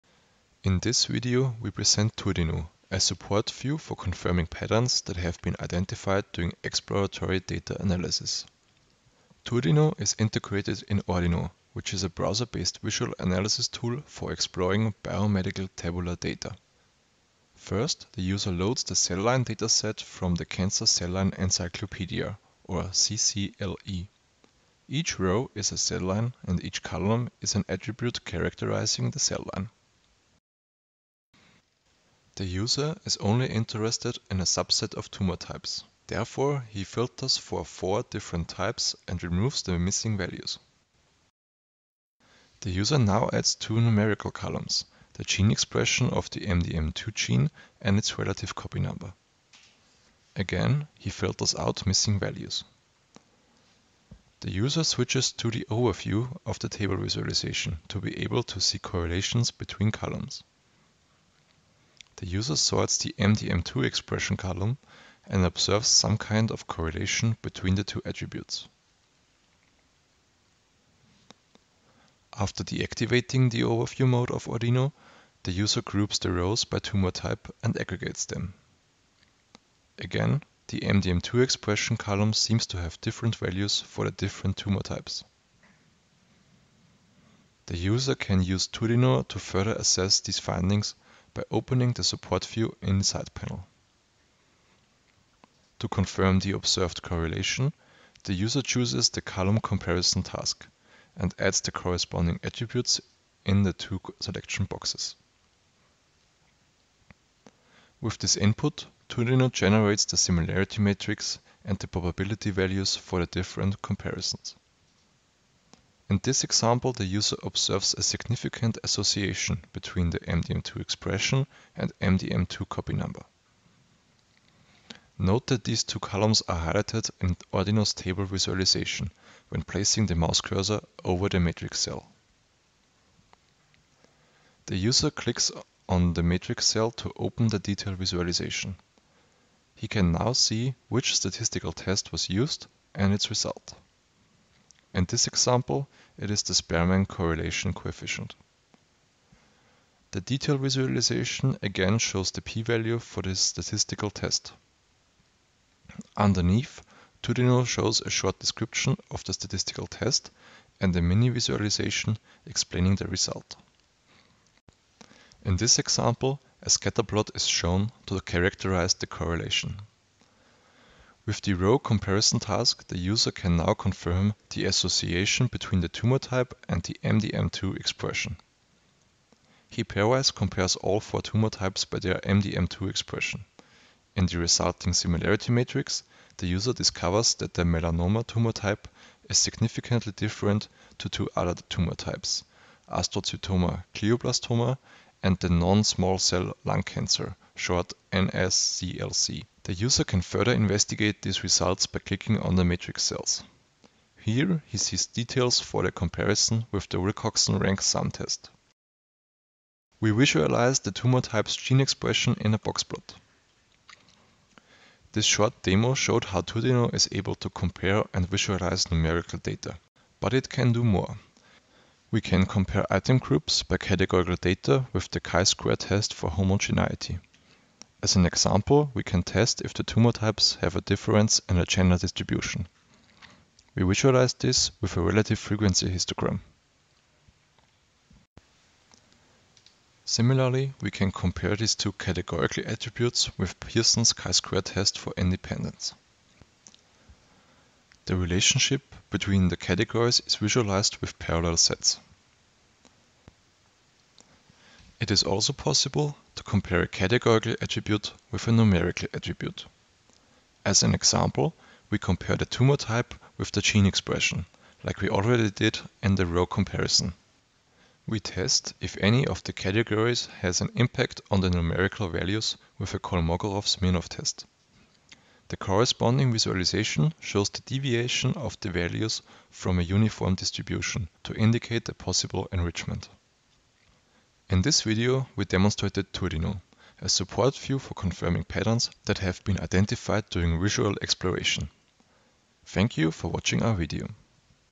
TourDino: A Support View for Confirming Patterns in Tabular Data
Authors:
Klaus Eckelt, Patrick Adelberger, Thomas Zichner, Andreas Wernitznig, Marc Streit

Abstract:
Seeking relationships and patterns in tabular data is a common data exploration task.
To confirm hypotheses that are based on visual patterns observed during exploratory data analysis, users need to be able to quickly compare data subsets, and get further information on the significance of the result and the statistical test applied.
Existing tools, however, either focus on the comparison of a single data type, such as comparing numerical attributes only, or provide little or no statistical evaluation to assess a hypothesis. To fill this gap, we present TourDino, a support view that helps users who are not experts in statistics to verify generated hypotheses and confirm insights gained during the exploration of tabular data.
In TourDino we present an overview of the statistical significance of various row or column comparisons. On demand, we show further details, including the test score, a textual description, and a detail visualization explaining the results.
To demonstrate the efficacy of our approach, we have integrated TourDino in the Ordino drug discovery platform for the purpose of identifying new drug targets.